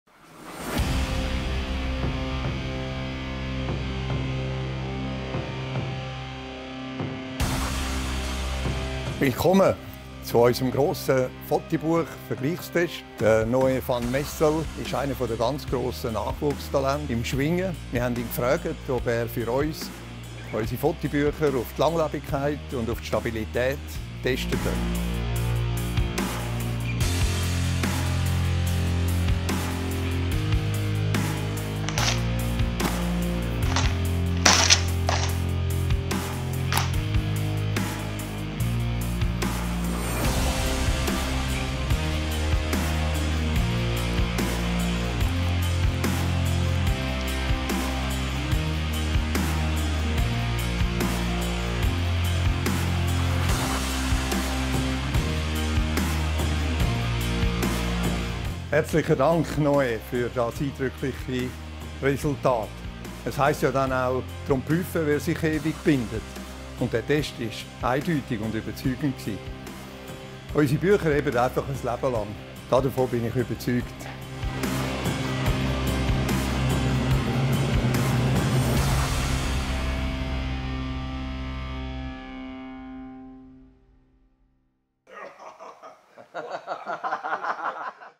The most unusual photo book comparison test.
For an independent photo book comparison test, we invited the strong young Swiss Kranzschwinger Noe van Messel to the Schwingkeller. Under the supervision of Hans Burkhardt, Chairman of the Board of Bookfactory by Bubu AG, he tests the stability and quality of conventional photo books with the photo books from Bookfactory.
Take a look for yourself.